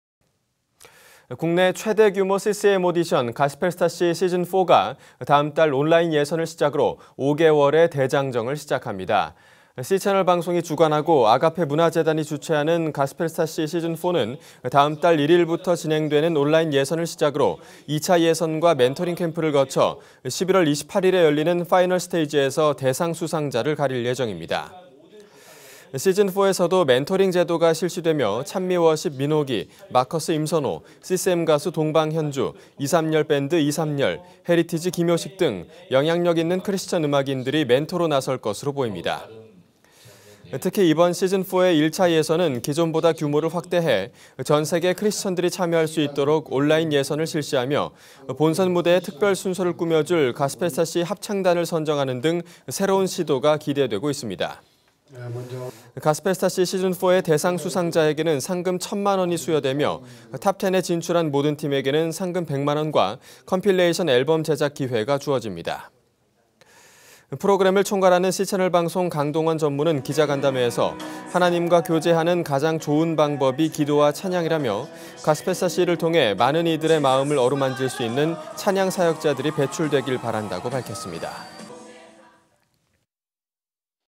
[C채널뉴스] 가스펠스타C 시즌4, 다음달 시작
국내 최대규모 CCM 오디션 가스펠스타C 시즌4가 다음달 온라인 예선을 시작으로 5개월의 대장정을 시작합니다.

C채널방송이 주관하고 아가페문화재단이 주최하는 가스펠스타C 시즌4는 다음달 1일부터 진행되는 온라인 예선을 시작으로 2차 예선과 멘토링 캠프를 거쳐 11월 28일에 열리는 파이널 스테이지에서 대상 수상자를 가릴 예정입니다.

시즌4에서도 멘토링 제도가 실시되며 찬미워십 민호기, 마커스 임선호, CCM가수 동방현주, 이삼열밴드 이삼열, 헤리티지 김효식 등 영향력 있는 크리스천 음악인들이 멘토로 나설 것으로 보입니다.

특히 이번 시즌4의 1차 예선은 기존보다 규모를 확대해 전 세계 크리스천들이 참여할 수 있도록 온라인 예선을 실시하며, 본선 무대의 특별 순서를 꾸며줄 가스펠스타C 합창단을 선정하는 등 새로운 시도가 기대되고 있습니다.

가스펠스타C 시즌4의 대상 수상자에게는 상금 1,000만원이 수여되며, TOP10에 진출한 모든 팀에게는 상금 100만원과 컴필레이션 앨범 제작 기회가 주어집니다.

프로그램을 총괄하는 C채널방송 강동원 전무는 기자간담회에서 "하나님과 교제하는 가장 좋은 방법이 기도와 찬양"이라며 "가스펠스타C를 통해 많은 이들의 마음을 어루만질 수 있는 찬양사역자들이 배출되길 바란다"고 밝혔습니다.